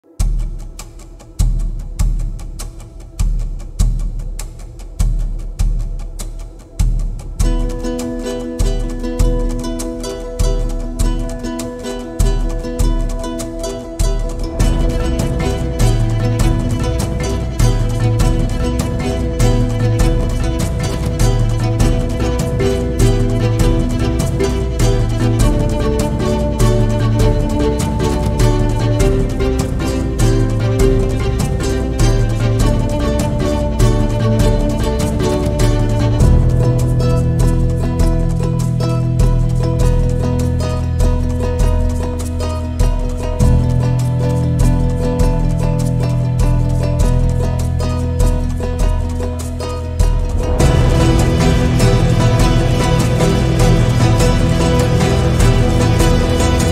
Drug Lord Gato Farfán Arrested In Colombia
One of the biggest cocaine traffickers in South America, Ecuadorian mobster Gato Farfán, was apprehended in Colombia.
Video of the arrest, which followed an 18-month joint investigation by Colombian and Ecuadorian police, as well as searches on multiple properties linked to Gato Farfán and the seizure of weapons and large sums of money, has been made public.

In collaboration with several Mexican cartels, Farfán, one of the continent's most wanted criminals, is said to have been in charge of a sizable drug shipment to the US at the time of his capture.